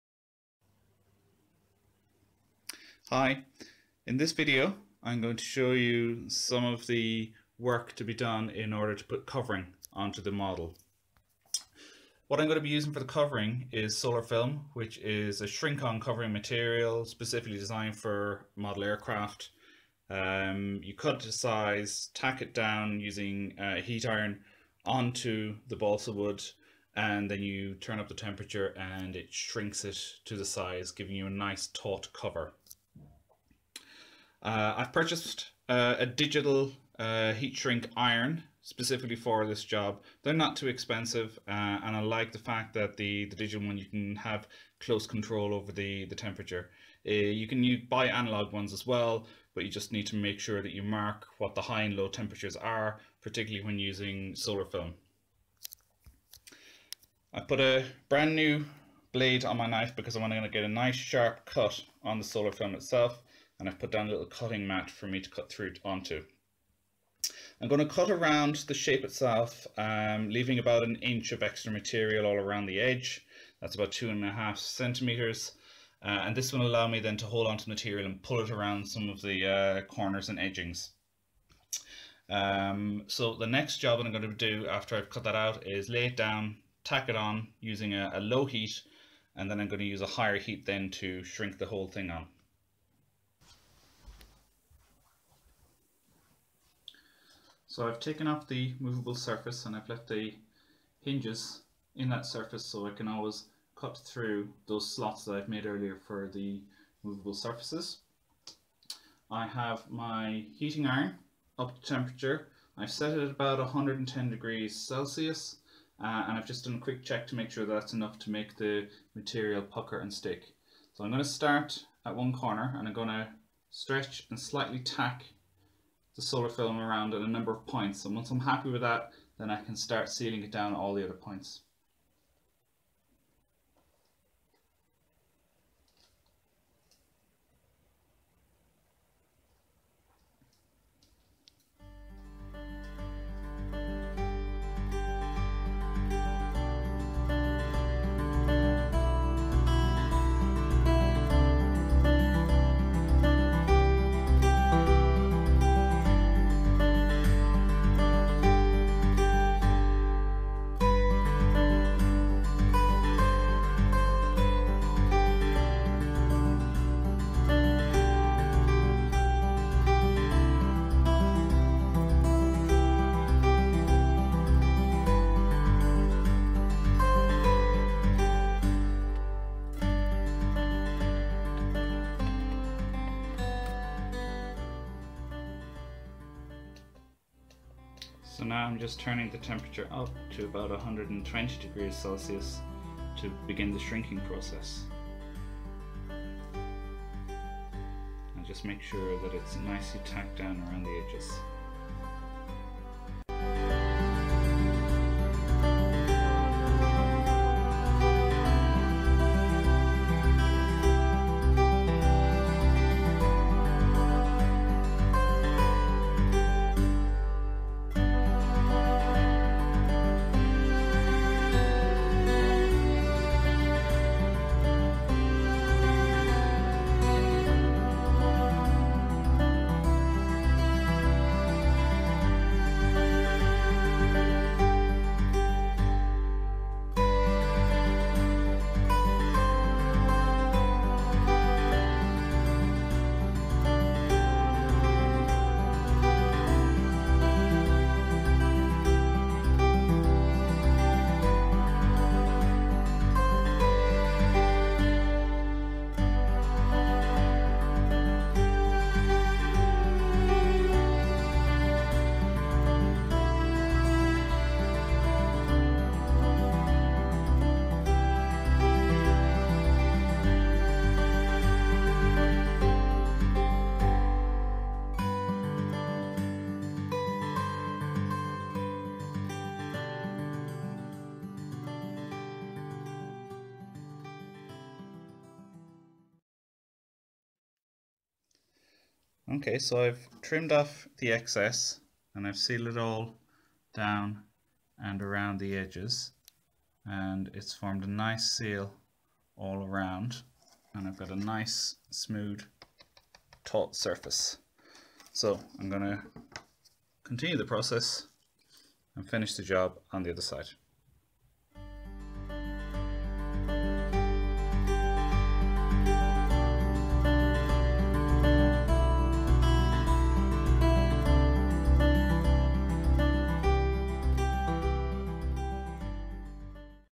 Video 13: Covering the RC plane using solarfilm
Video 13 of the Millie Bob build - Covering the plane using Solarfilm. This video demonstrates the covering of the horizontal stabiliser, and the same techniques apply to the rest of the plane.

One piece of advice: TAKE YOUR TIME! Be patient, don't worry about every single wrinkle as the shrinking will remove them, and slowly complete the job - never rush it!